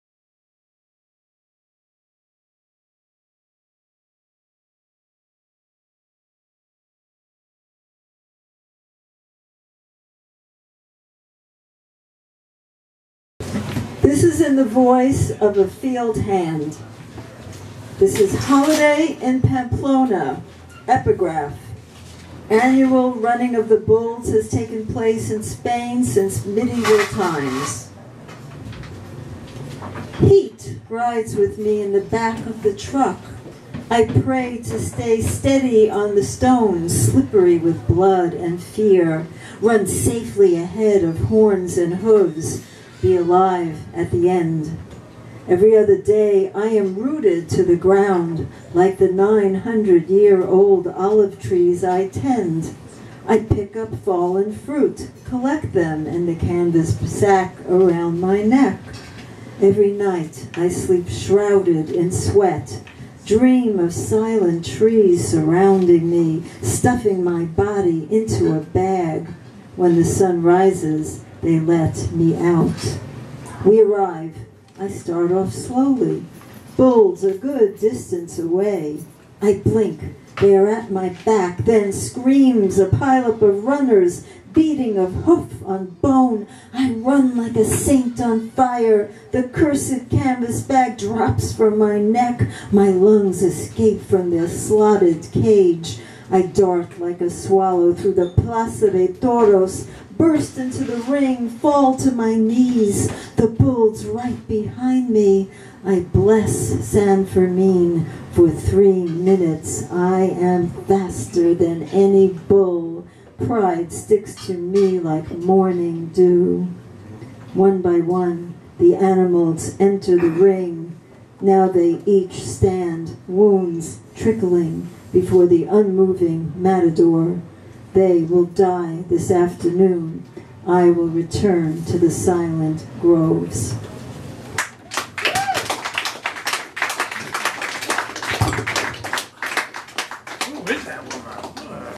"Silver Tongued Devil" Madeline Artenberg for Rimes of The Ancient Mariner November 30, 2016
"Silver Tongued Devil" Madeline Artenberg for Rimes of The Ancient Mariner November 30, 2016 @ Three of Cups Lounge.